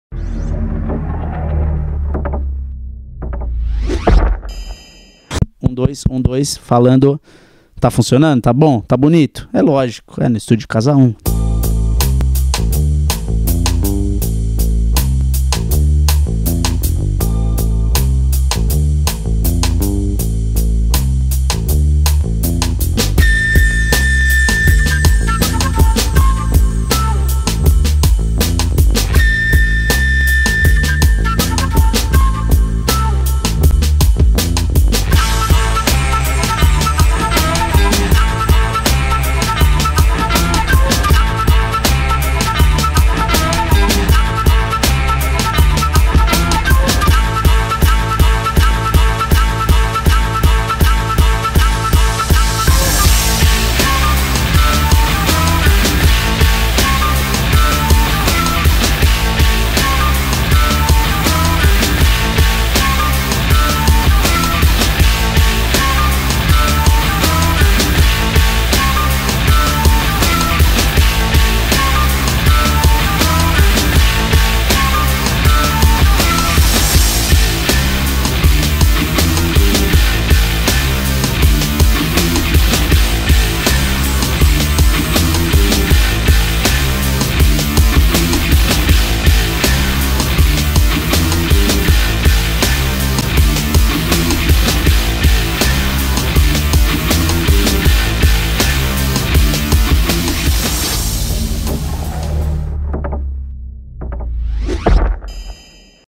CASA 1 Studio - Produção Musical - Gravações
Video com diversos artistas que já passaram pelo studio.